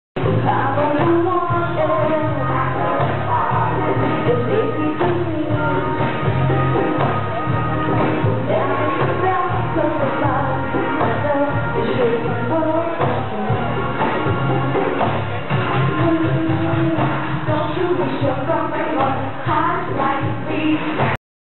Pussy Cat Dolls One Woman Show
Pussy Cat Dolls one woman show at Bangkok Suan Lum Night Bazaar